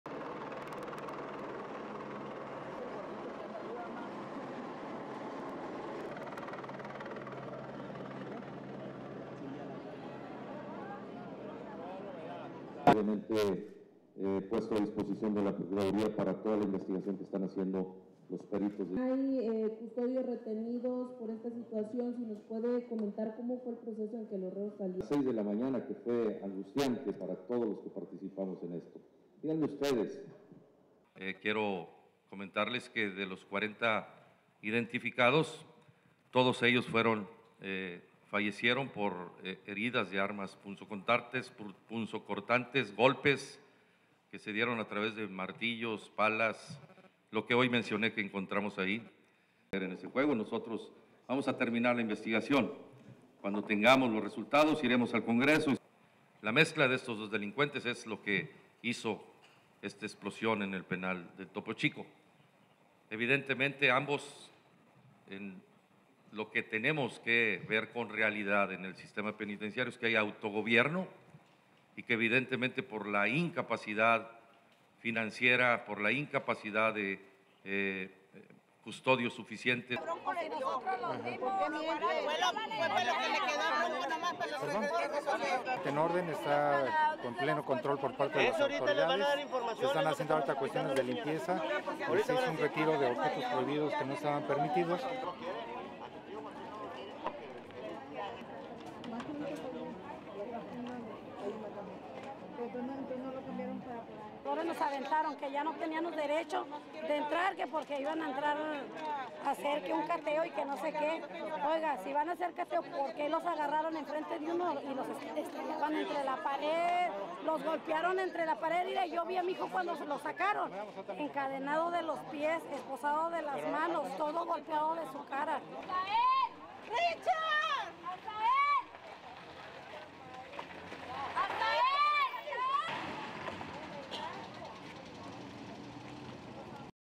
Prison back under control in Mexico
(12 Feb 2016) RESTRICTION SUMMARY: AP CLIENTS ONLY

Monterrey, Mexico - 12 February 2016
1. Various of helicopter flying over the Topo Chico, where a fight between factions left 49 dead
2. Cameramen waiting for press conference of Nuevo Leon state governor Jaime Rodriguez
3. Various of Rodriguez holding news conference
4. SOUNDBITE (Spanish) Jaime Rodriguez, Nuevo Leon State Governor:
"I would like to tell you that of the 40 identified, all of them died from wounds from stabbing and cutting weapons, blows from hammers and clubs. From what I mentioned we found there today (inside the prison)."
5. Rodriguez holding news conference
6. SOUNDBITE (Spanish) Jaime Rodriguez, Nuevo Leon State Governor:
"The mixing of those two delinquent (organisations) is what caused this explosion inside the Topo Chico prison. Evidently both of them. What we have to do is see the reality of the penitentiary system and that they are self-governed and due to the lack of financial capability (to pay for prison) and not enough guards."
7. National Human Rights Committee representative Benjamin Vergara speaking to relatives of inmates outside Topo Chico prison
8. SOUNDBITE (Spanish) Benjamin Vergara, National Human Rights Committee representative:
"The prison is in order. The authorities are fully in control. They are cleaning the facility and taking out the objects (weapons) that were not allowed."
9. Various of relatives watching list reading (Spanish) "Transfers from the prison"
10. SOUNDBITE (Spanish) Antonia Barron, mother of an inmate:
"Now they threw this at us that we did not have the right to go in. That they are going inside and doing a search and I don't know what. Listen, if they are going to do that why did they grab them in front of us and throw them against the wall. They (police) threw them (prisoners) against the walls. Look, I saw my son when they removed him and his feet were chained and his hands were in handcuffs and his face was all beaten up."
Monterrey, Mexico - 11 February 2016
++NIGHT SHOTS++
11.  Ambulance carrying inmates leaving the Topo Chico prison
12. Truck leaving prison
STORYLINE:
Mexico's deadliest prison brawl in years was a bloodbath in which inmates attacked each other with hammers, cudgels and makeshift blades, authorities said on Friday, underlining yet again the power that drug cartels wield inside many of the country's prisons.
Jaime Rodriguez, governor of the northern state of Nuevo Leon, said 60 hammers, 86 knives and 120 shivs were used in the previous day's fighting at the Topo Chico prison in Monterrey, where 49 inmates were hacked, beaten or burned to death, and a dozen more injured.
At least 40 of the victims "died from wounds from stabbing and cutting weapons, blows from hammers and clubs," Rodriguez said at a news conference in Monterrey on Friday.
Authorities also seized various kinds of contraband items from marijuana and cocaine to televisions and USB memory sticks.
A dispute between rival factions of the Zetas cartel was believed to be behind the violence.
He acknowledged that prisoners effectively lord over the facility and that there were not enough guards watching them.
About half the inmates at Topo Chico have been sentenced for minor offenses or are suspects still awaiting trial.
Nevertheless they are housed in the prison's overcrowded general population alongside many of the country's most hardened killers.
Relatives of inmates still gathered on Friday outside the prison to know if their family members were being transferred to a different person and visit them.